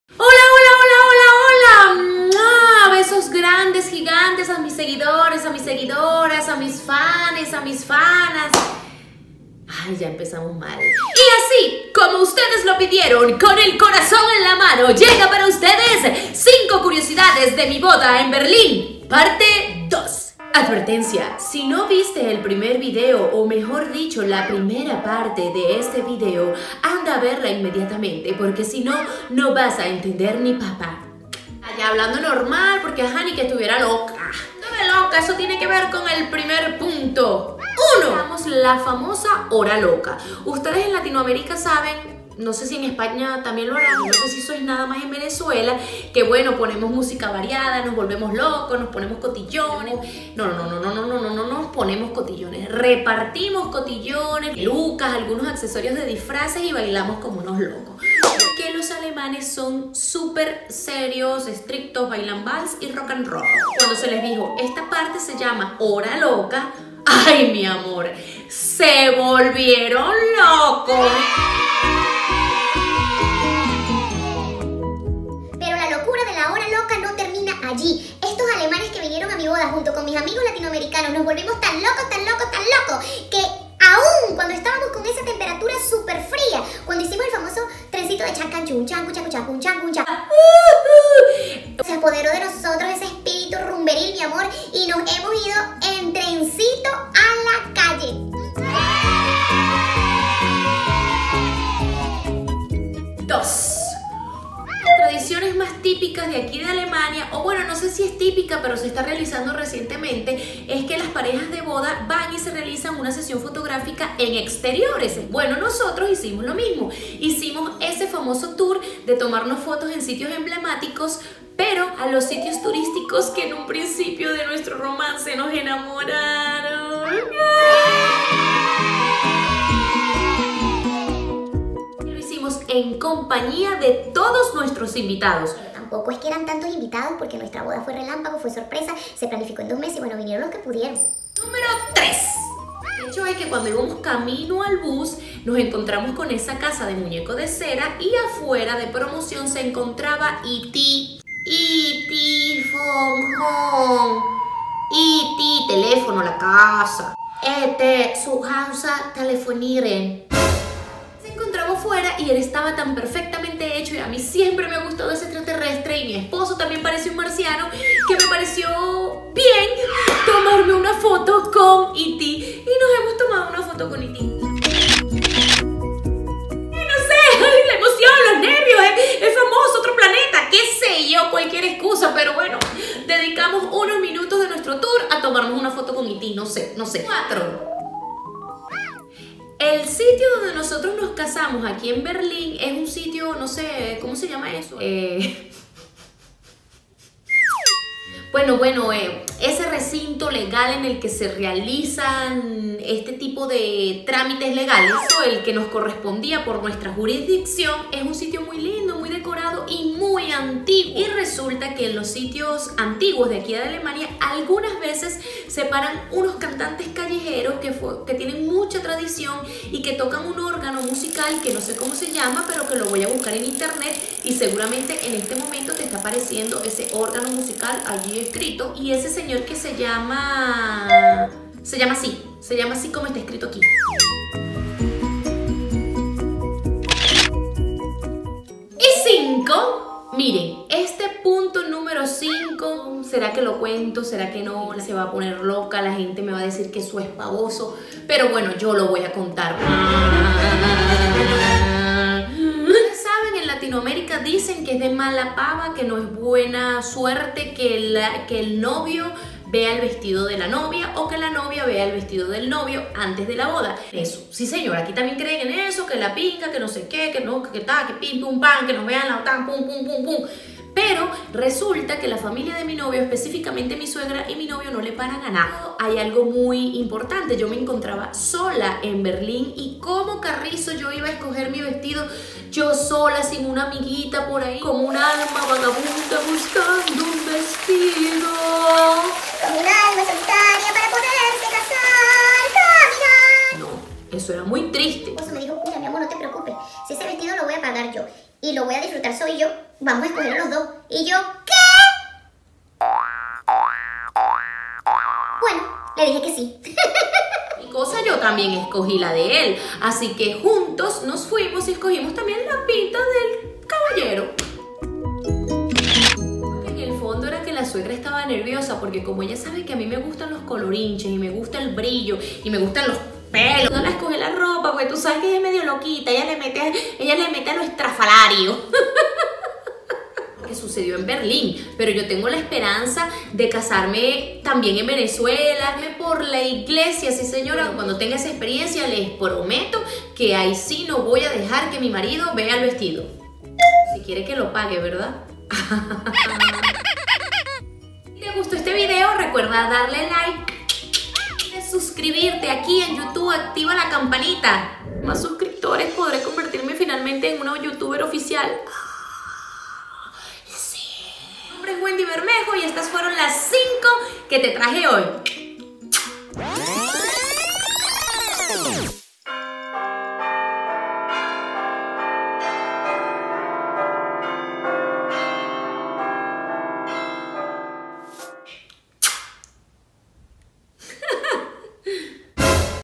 UNA BODA DIFERENTE EN BERLÍN / PARTE 2 (🖐🏻Conteo #5)
Acerca de mi boda en Berlin, aquí les dejo MÁS CURIOSIDADES.

Las fotos fueron tomadas por la talentosisima fotógrafa venezolana BRI DIEZ. En instagram @bridiez_photo

Para entender mejor todo el cuento los invito a ver la primera parte de éste video:

¡LOS QUIERO!